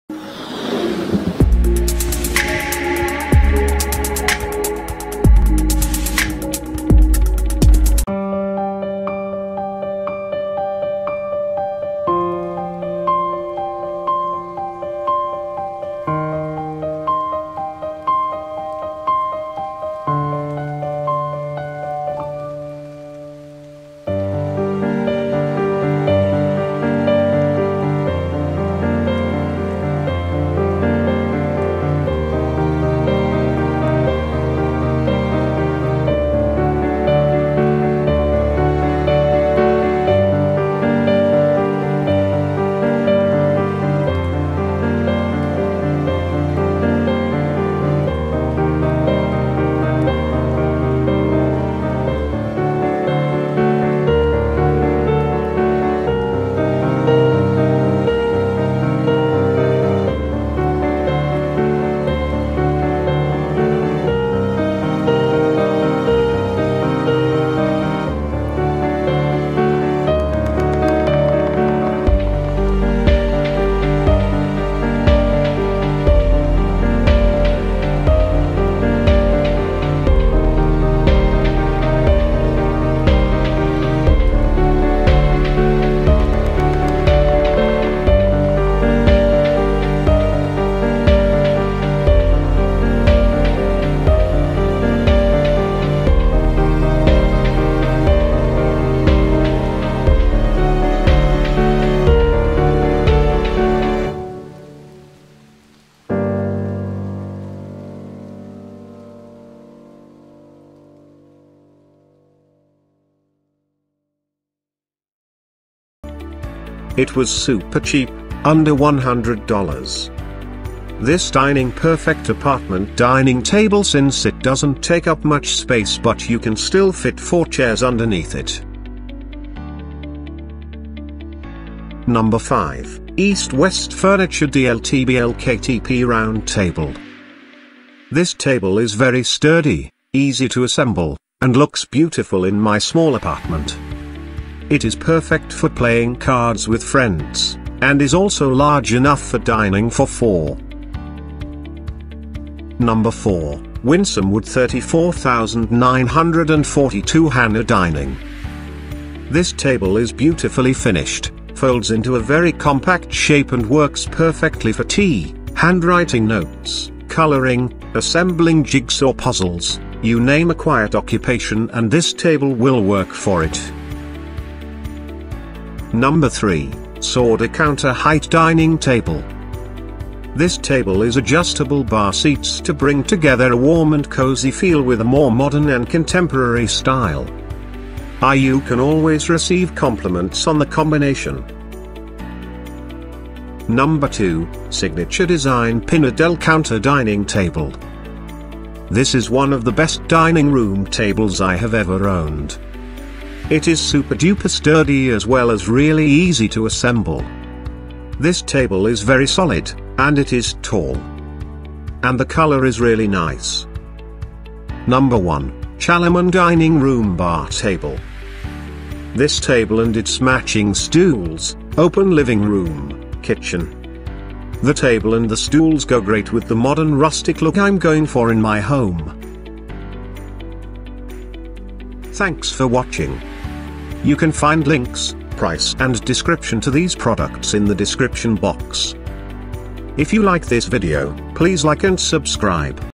Top 10 Best Dining Tables | Buy on Market Dining Tables for Every Space
Top 10 Best Dining Tables | Buy on Amazon Dining Tables.

Hi guys, I am going to show video reviews on the Top 10 Best Dining Tables on the market. Get your product from here ✅

Top 10 Best Dining Tables Links: ✅

► 10). Clayton Rectangular Dining Table Cappuccino.

► 9). Zinus Modern Studio Collection Soho Dining Table.

► 8). Nathan James Kalos Solid Wood Drop Leaf Folding Kitchen Dining.

► 7). Winsome Wood Groveland Dining.

► 6). Coavas Chad White Round Modern Dining Table.

► 5). East West Furniture DLT-BLK-TP Round Table.

► 4). Winsome Wood 34942 Hannah Dining.

► 3). Sauder Counter Height Dining Table.

► 2). Signature Design Pinnadel Counter Dining Table.

► 1). Challiman Dining Room Bar Table.